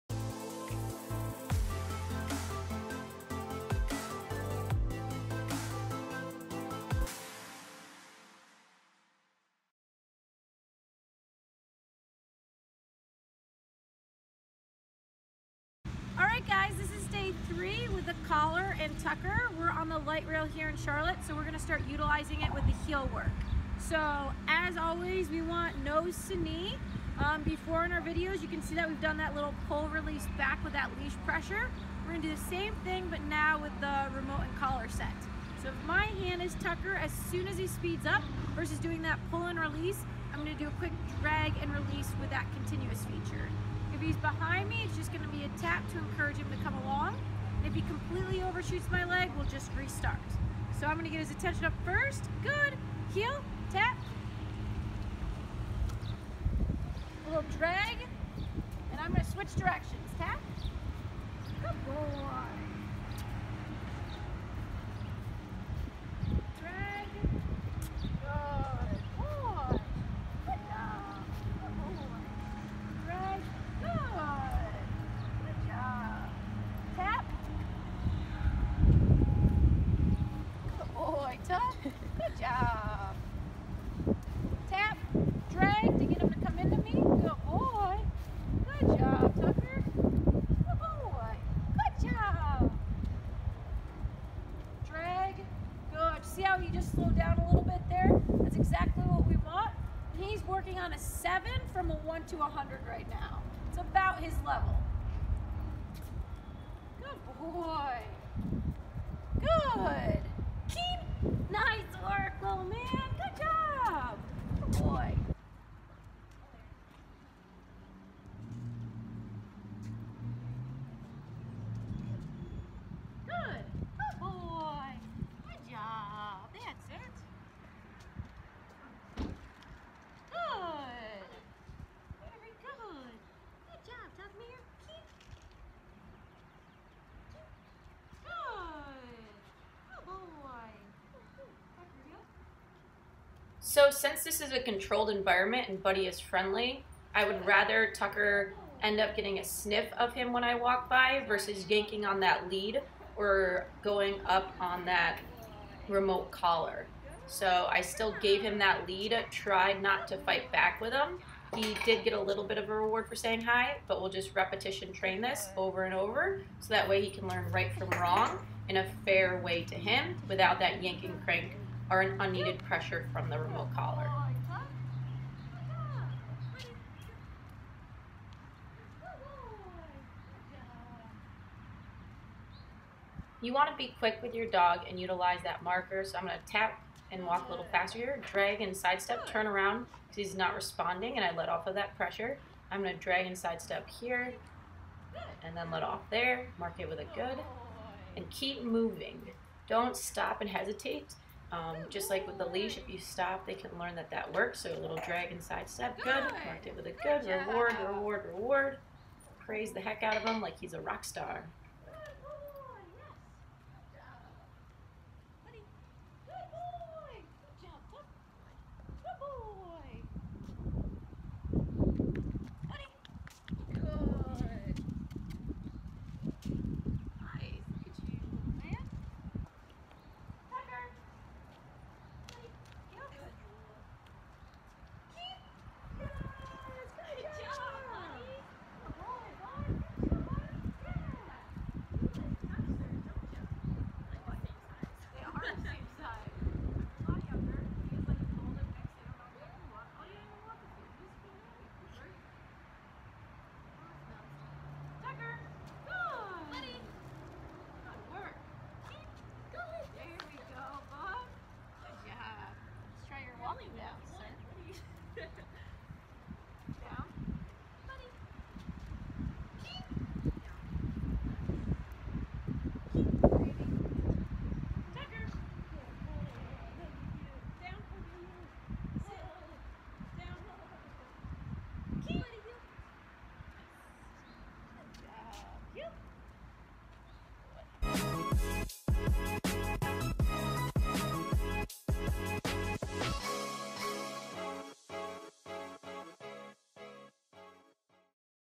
Golden Retriever Puppy Tucker: Day 44
Day 3 of the e collar being added to Tucker's training, working on the heel.